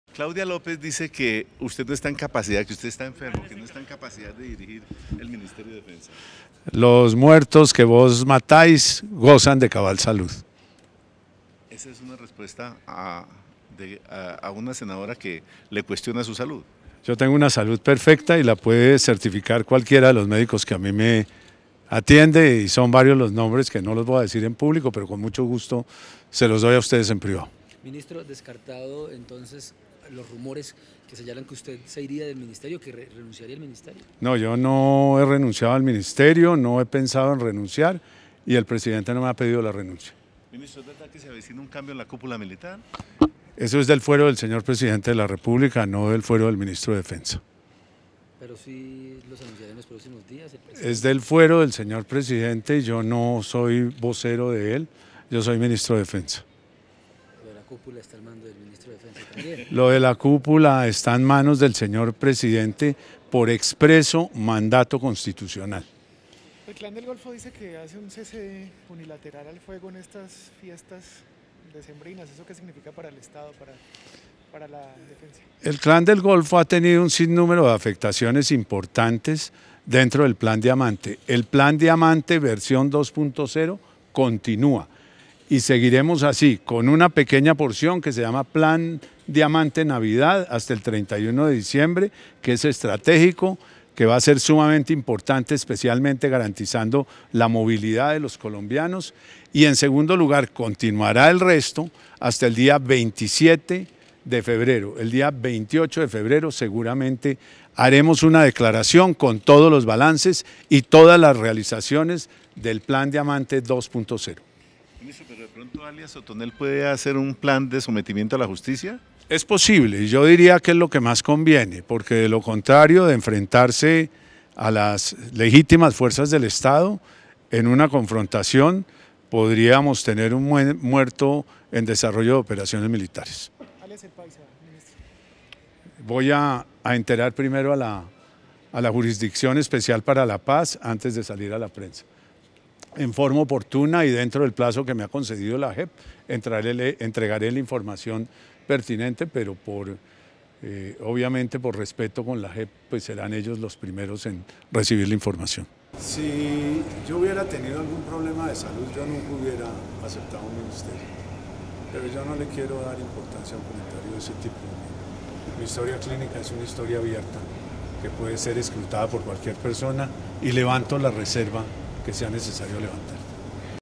Declaciones de Mindefensa
"No habría aceptado ningún cargo público si no gozara de buena salud. Mi compromiso con el país y con el Presidente Iván Duque se mantiene al frente del mindefensa. Los muertos que vos matáis gozan de cabal salud": Guillermo Botero, ministro de Defensa